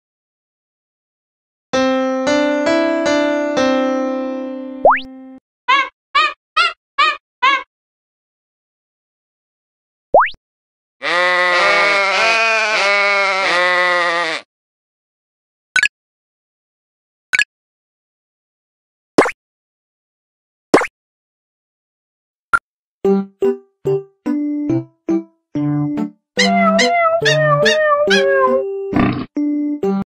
Animal Piano Mix - Music Composition & Mixing & File Export
Animal Piano Mix encourages children to compose music with 12 animal voices in 36 pitches, helping to unlock and boost children limitless potential in creativity.